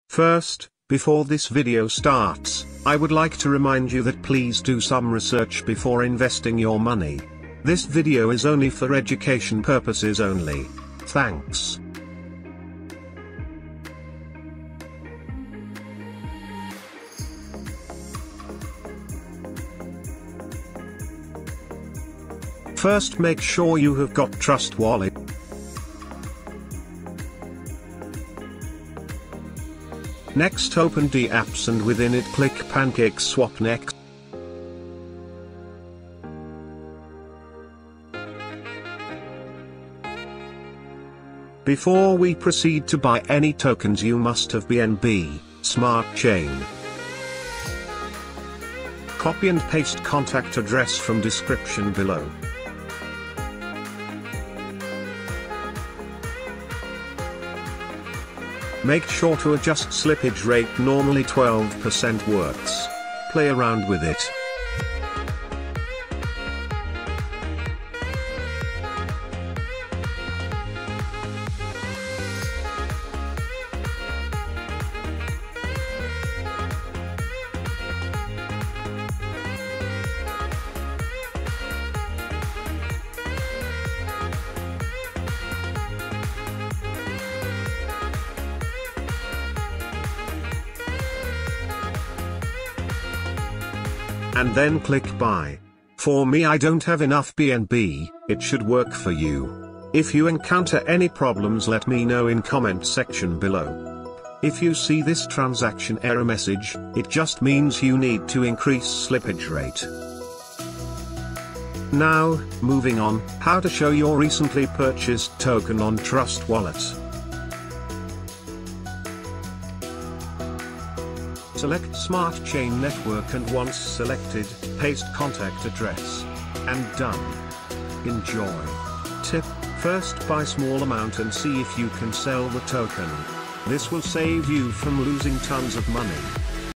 How to Buy and show Shiba Hex Pulse Token on Trust Wallet [Full Guide]
SHIBA HEX PULSE Is a community driven project on the Binance Smart Chain network. It's a rewards token, where holders benefit from guaranteed price appreciation over time due to the deflationary tokenomics

Video covers:
How to buy shiba hex pulse token on Trust Wallet via pancakeswap Full Guide,
setting slippage for shiba pulse token ,
how to show Shiba pulse token on Trust Wallet,
solving pancakeswap transaction gas Error

Contact Address: 0x62b6437349c0238d05acefeb0c4a14e9c8ea0f8d